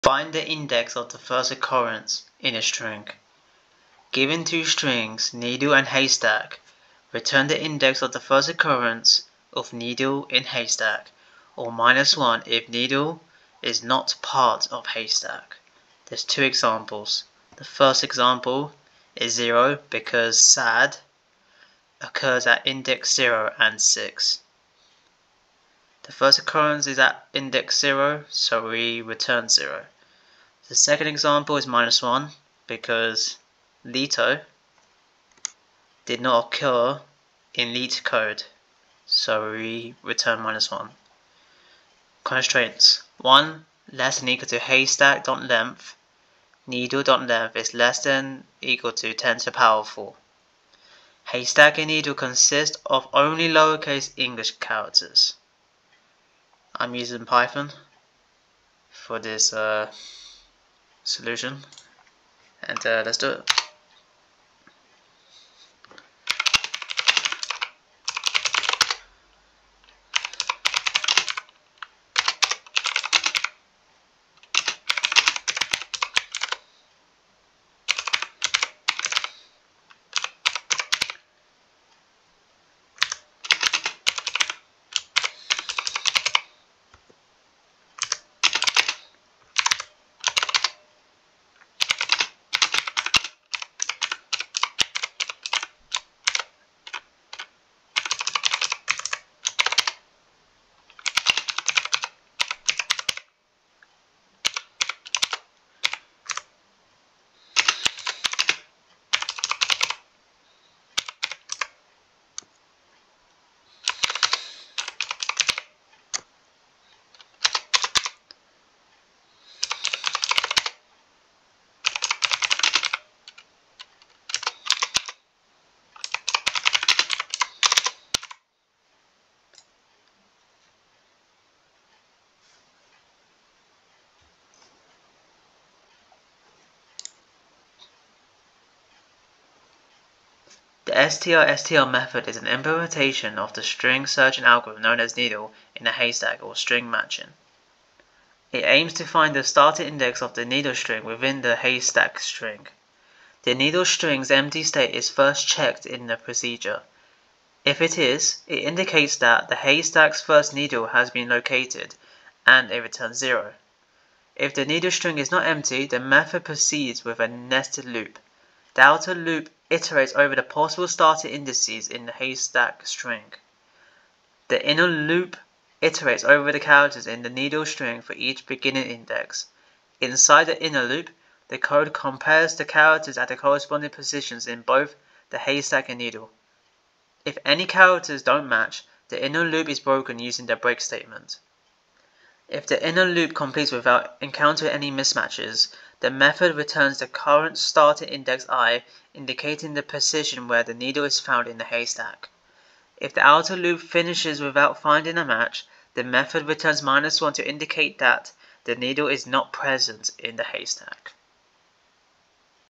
FIND THE INDEX OF THE FIRST OCCURRENCE IN A STRING
Given two strings needle and haystack, return the index of the first occurrence of needle in haystack, or -1 if needle is not part of haystack.

EXPLANATION CODE:
1) The strStr method is an implementation of the string searching algorithm known as "needle in a haystack" or string matching. It aims to find the starting index of the needle string within the haystack string.

2) The needle string's empty state is first checked in the procedure. If it is, it indicates that the haystack's first needle has been located, and it returns 0.

3) If the needle string is not empty, the method proceeds with a nested loop. The outer loop iterates over the possible starting indices in the haystack string.

4) The inner loop iterates over the characters in the needle string for each beginning index.

5) Inside the inner loop, the code compares the characters at the corresponding positions in both the haystack and needle. If any characters don't match, the inner loop is broken using the break statement.

6) If the inner loop completes without encountering any mismatches (i.e., all characters match), the method returns the current starting index i, indicating the position where the needle is found in the haystack.

7) If the outer loop finishes without finding a match, the method returns -1 to indicate that the needle is not present in the haystack.

Contents:
0:00 - Reading
1:05 - Solving
2:20 - Explaining